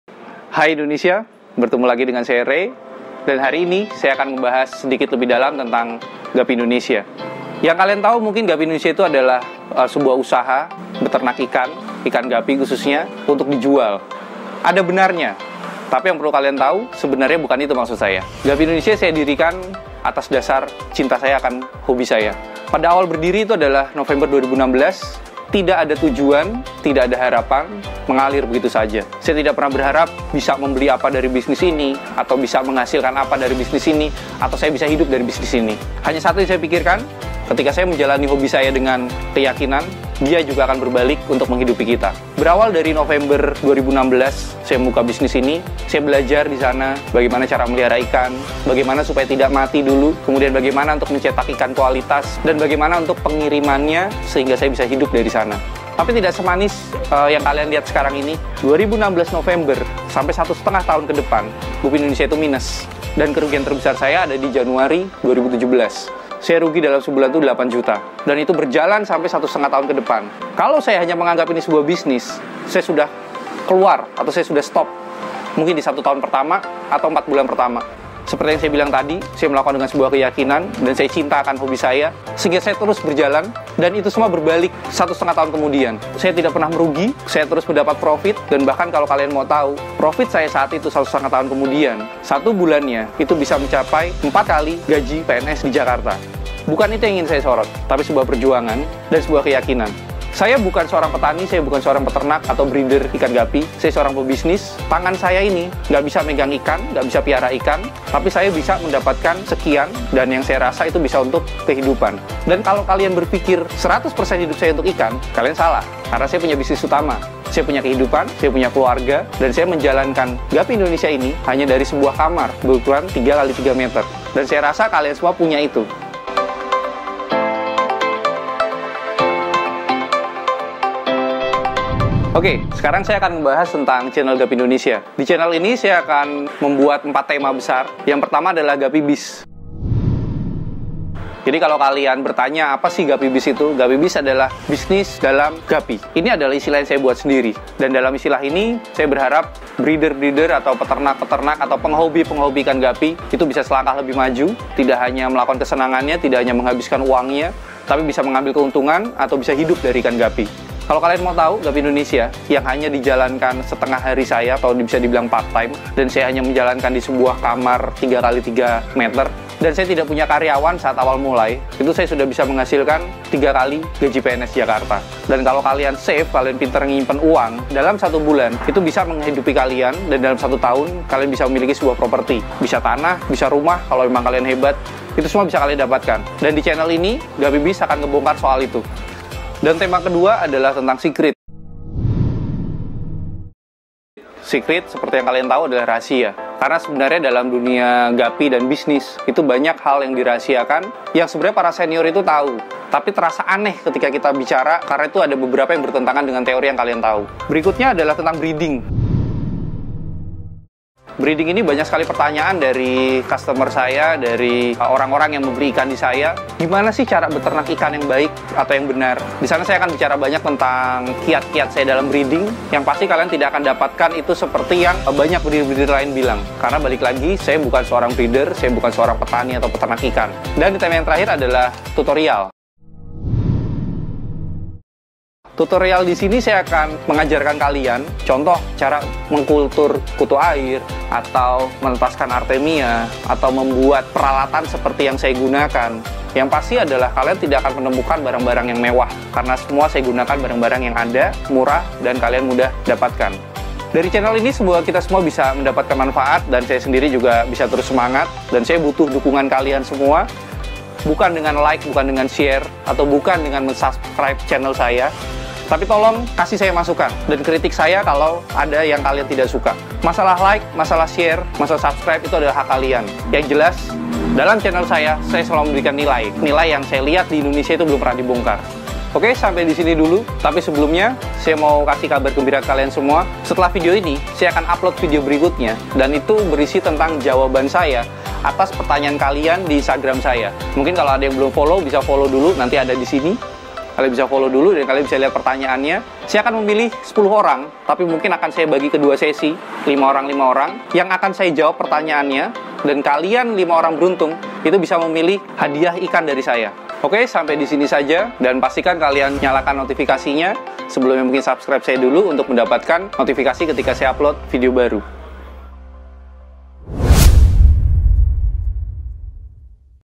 TANPA BAKAT PROFIT 3X GAJI PNS JAKARTA. Bisnis Ikan Guppy
Saya yang tidak berbakat beternak Ikan Guppy saja bisa, Apalagi kamu
Share pengalaman kalian beternak dan berbisnis ikan Guppy di kolom komentar ya

Di video ini saya juga menjelaskan sedikit tentang cara packing ikan Guppy yang akan dikirim ke Luar kota mengunakan Tiki, Kalau ada yang ingin kamu tanyakan, bisa komen dibawah

Selamat menikmati sambil belajar

UNTUK ORDER PRODUK GUPPY INDONESIA HUBUNGI